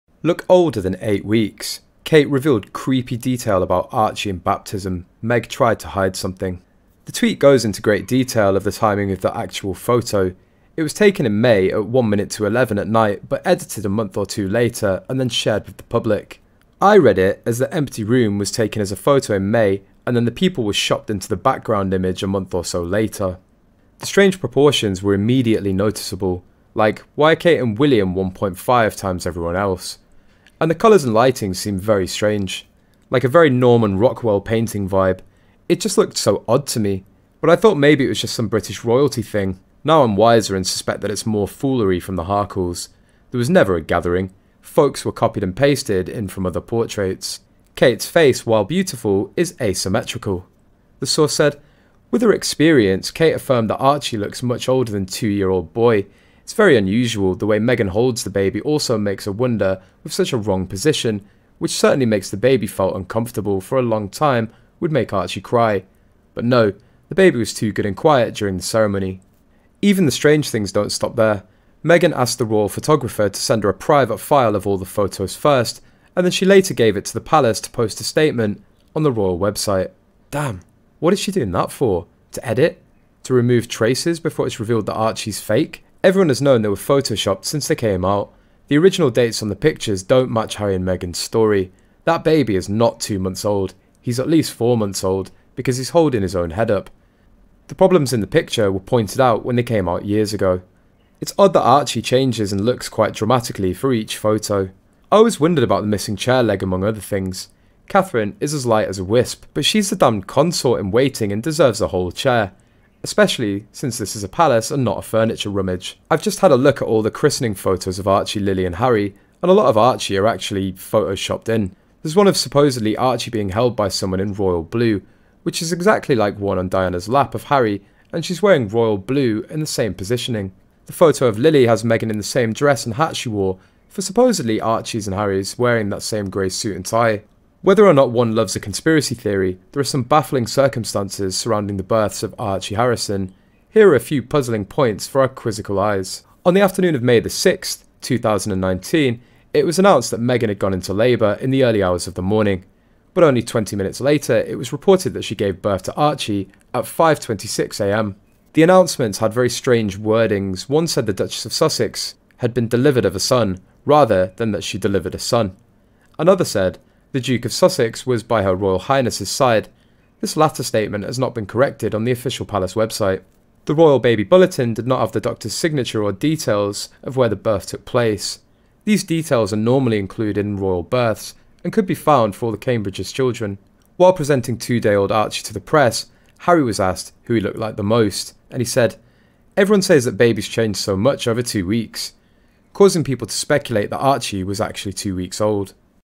LOOK OLDER THAN 8 WEEKS Kate Revealed Creepy Detail About rchie In Baptism Meg Tried To Hide
The strange proportions were immediately noticeable (like why are Kate and William 1.5x everyone else), and the colors and lighting seemed strange. Like a very Norman Rockwell painting vibe. It just looked so strange to me, but I thought maybe it was just some British royalty thing lol. Now I’m wiser and suspect that it’s more foolery from the Harkles. There was never a gathering, folks were copy pasted in from other portraits.
Kate's face, while beautiful, is asymmetrical.
The source said, with her experience, Kate affirmed that Archie looks much older than 2 year old boy, it is very unusual, the way Meghan holds the baby also makes her wonder with such a wrong position, which certainly makes the baby felt uncomfortable, for a long time would make Archie cry, but no, the baby was too good and quiet during the ceremony. Even, the strange thing does not stop there, Meghan asked the royal photographer to send her a private file of all the photos first and then she later  gives it to the palace to post a statement with the photo on the royal website. Damn, what is she doing that for? to edit, remove traces before it is revealed that Archie is fake?